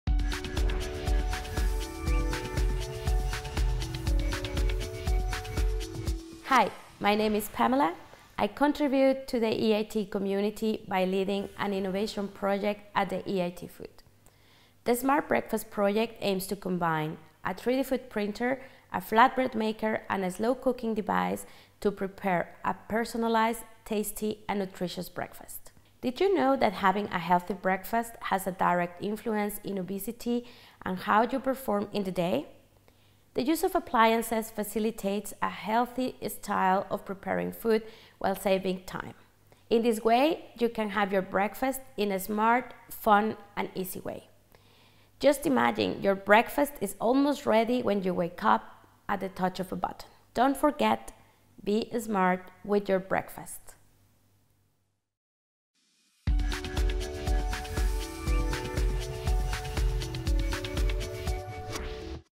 Pamela Vazquez - 2018 EIT Woman Award nominee
Pamela Vazquez, Chief Food Technology Officer at Flatev AG, is one of the nominees for the 2018 EIT Woman Award.

Find out more about the 2018 EIT Awards